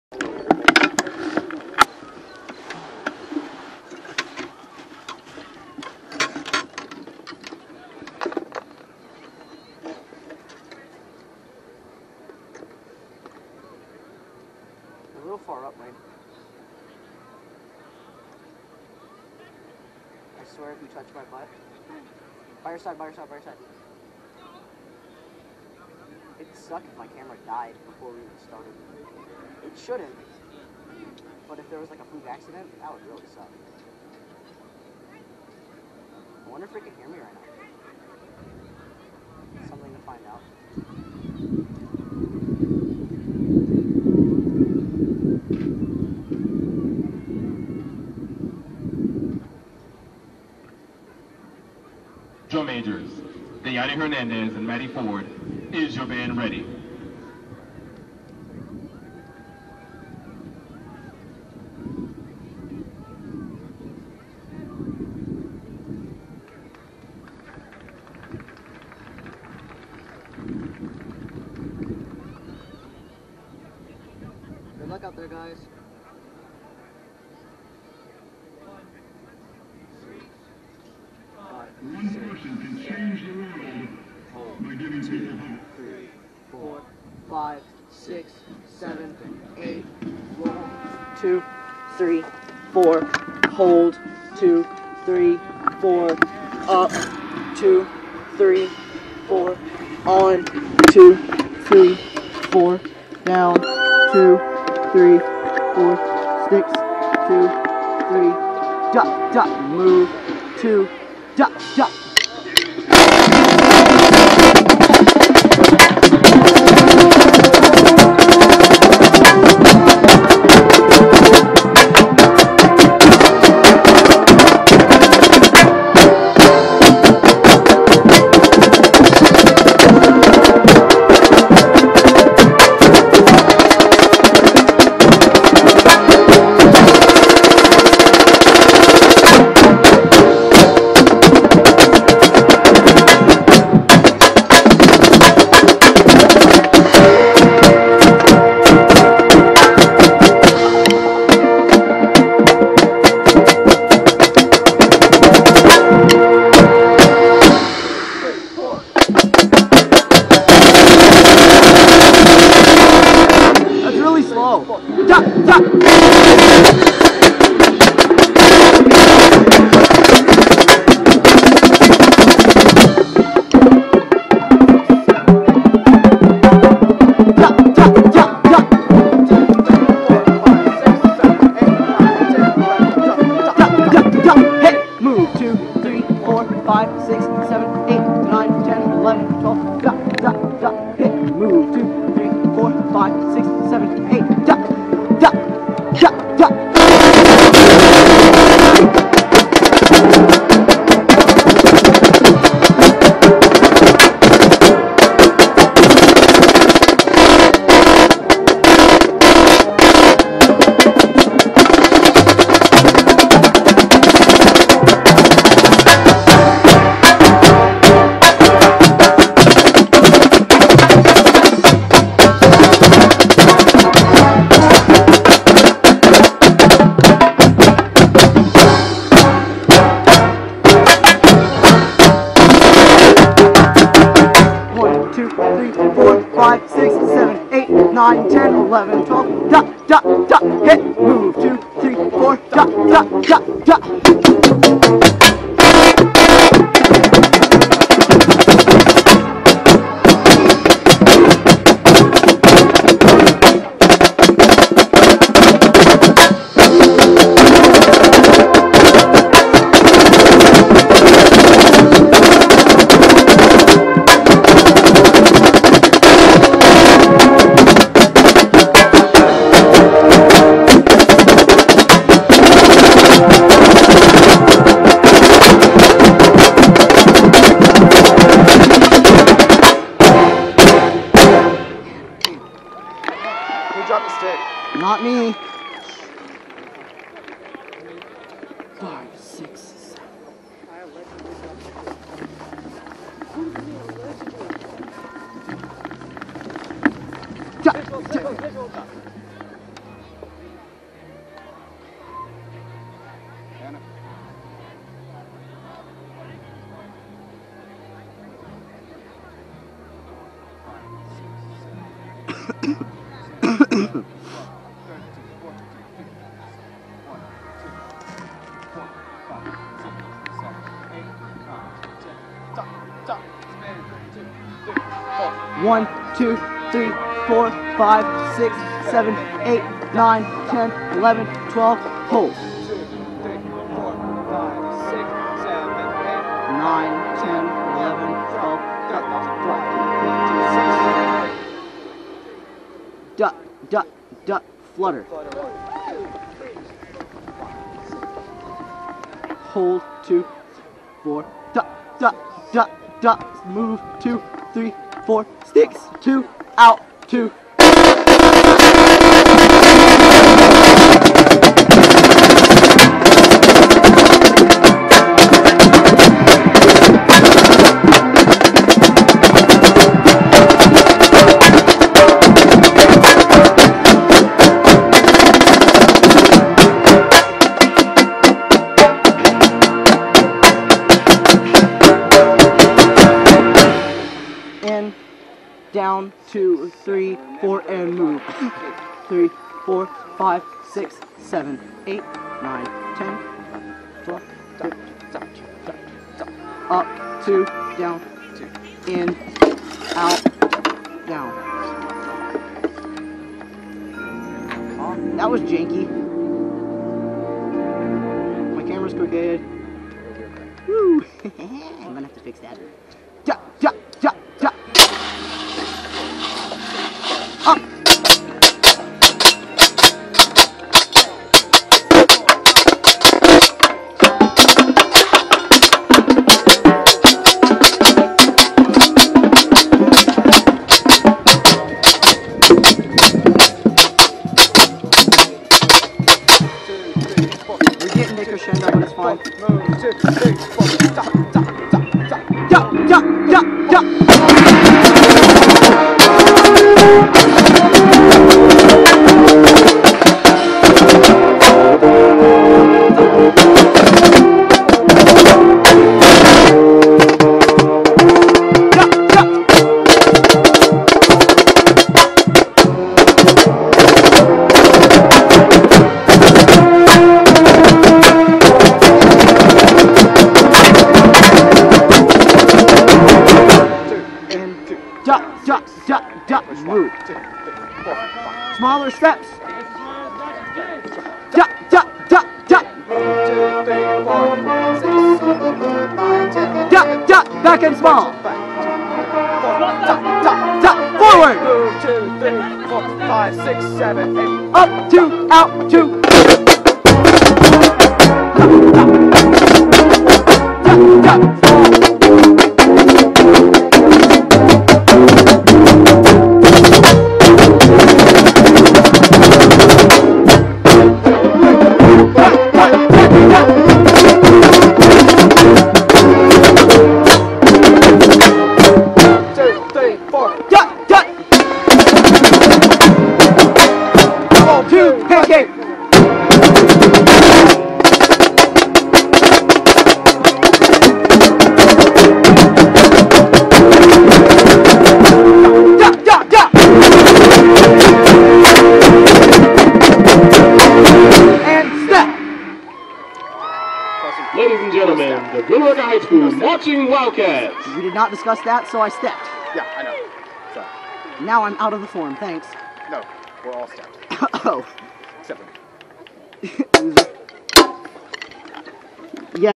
2022 Marching show "Call To Action" snare cam but the camera is really cheap
I don't remember how i had the camera set up, or even if i had the mic plugged in or not. I have since lost the mic i used here, and broken all of the mounting equipment. I hope to find a better setup b4 the season ends this year XD